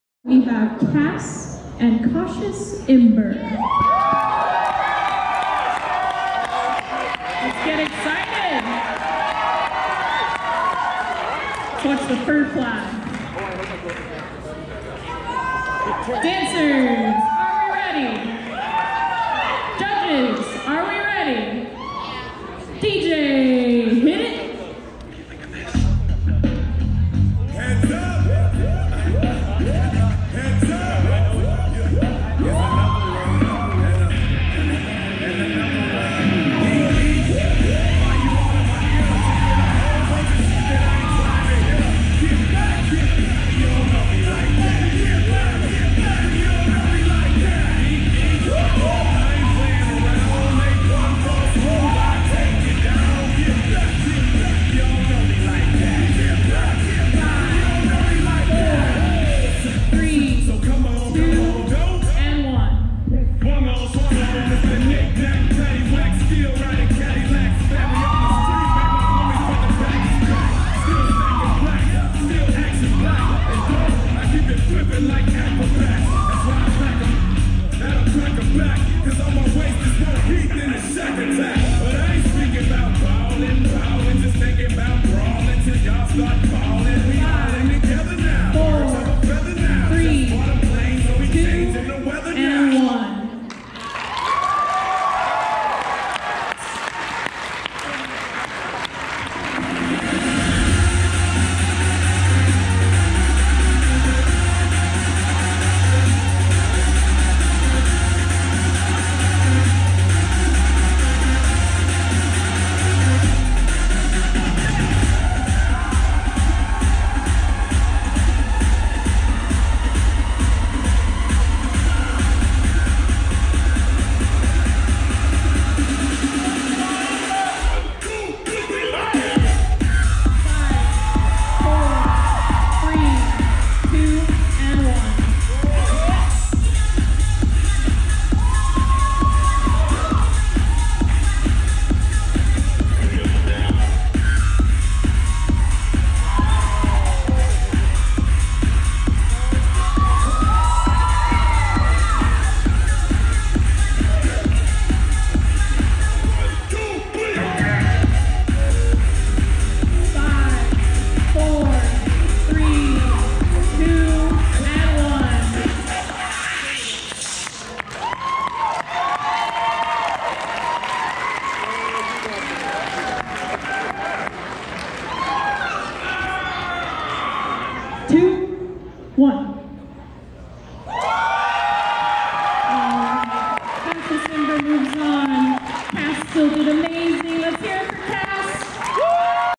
Denfur 2019 Rocky Mountain Rumble: Fursuit-1
Dance Battle Event aka Floor Wars
Fursuit Category:
Cautious Imber vs Cass